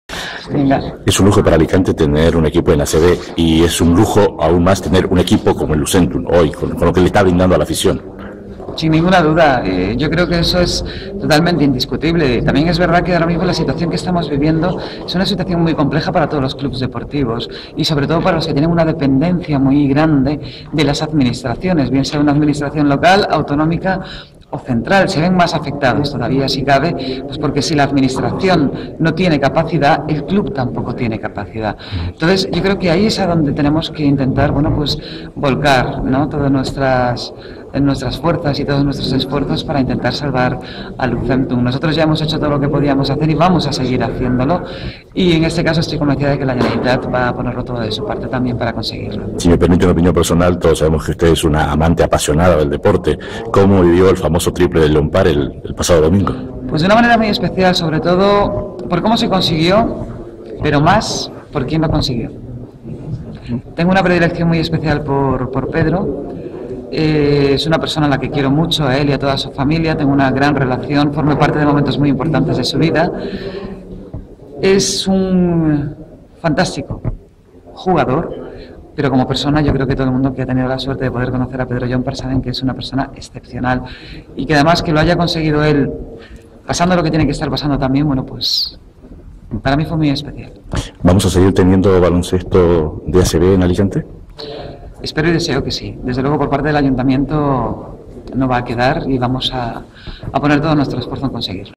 120403 - SONIA CASTEDO HABLA DEL LUCENTUM ALICANTE.flv
Sonia Castedo - Alcaldesa del Ayto de Alicante - dialogó con Teddy García en una entrevista especial de NCI. En la ocasión, Castedo habló del Lucentum Alicante y del jugador Pedro Llompart.